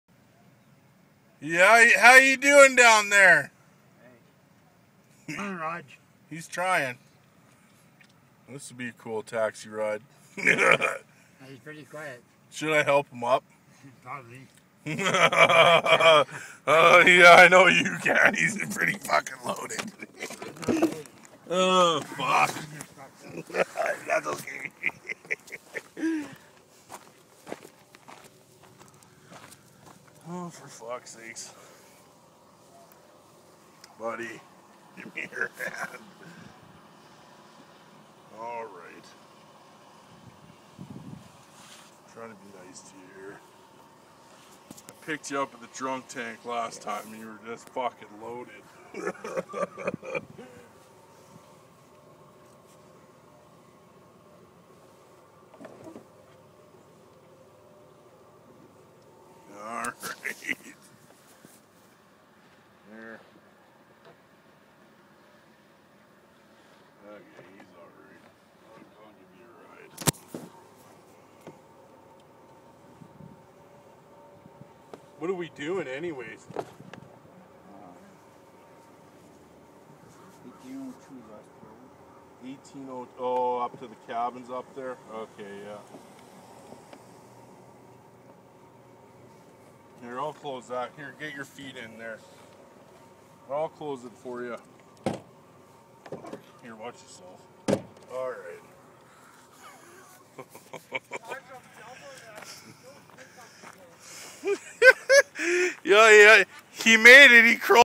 GUY WANTED MORE BEER AND HE CRAWLED TO THE TAXI REPEAT CUSTOMER.....HOLY FUCK CRAZIEST CAB FARE EVER
CRAZIEST FARE LOL I COULD HAVE JUST LEFT HIM THEIR AND DROVE AWAY , WHAT CAB DRIVERS ARE AS BRAVE AS I AM TO TAKE PEOPLE LIKE THIS.